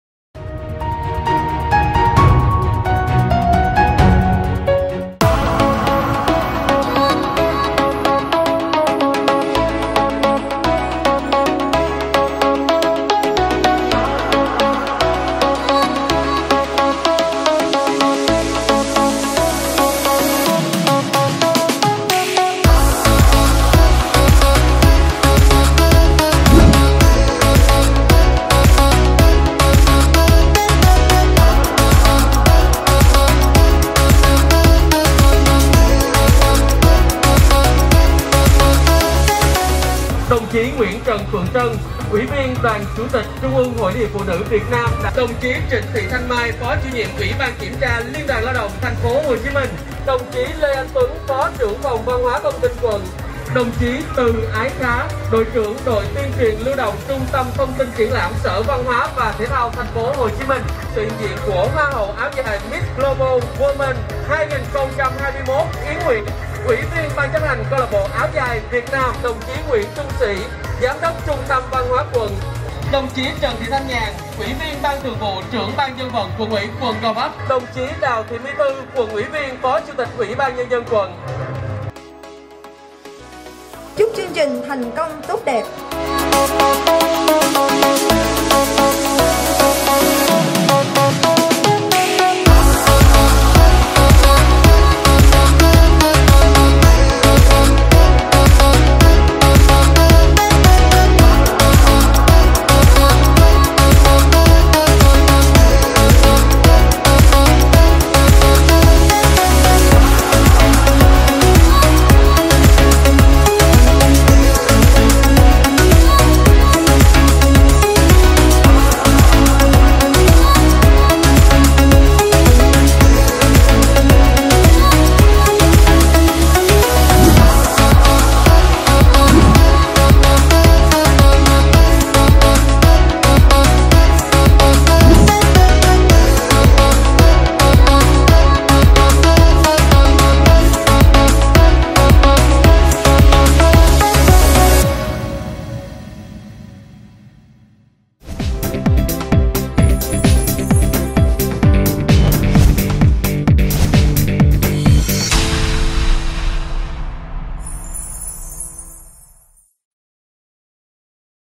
CHƯƠNG TRÌNH TRÌNH DIỄN ÁO DÀI TẠI KĐT CITYLAND PARK HILLS
Với chủ đề “Áo dài Việt Nam - Tôn vinh vẻ đẹp Việt” năm 2022, sự kiện trình diễn trang phục áo dài kết hợp nhạc nước đã mang đến những cung bậc cảm xúc thú vị và mới mẻ cho khách tham quan.

Bên cạnh phần trình diễn trang phục áo dài trên nền của ánh sáng, nhạc nước kết hợp cùng âm nhạc, trống, kèn saxophone, sự góp mặt của các tiết mục cổ động, võ thuật…, mang đến không gian đậm chất nghệ thuật và thư giãn dịp cuối tuần.

Chương trình được tổ chức bởi Hội Liên hiệp Phụ nữ - Liên đoàn Lao động quận Gò Vấp – Phòng VHTT Quận Gò Vấp với sự đồng hành của Nhà Tài trợ - Công ty TNHH Đầu tư Địa Ốc Thành Phố CityLand và Công ty TNHH Touch Music.

📣 Theo dõi fanpage CityLand Group để liên tục cập nhật những hoạt động mới nhất của CityLand nhé!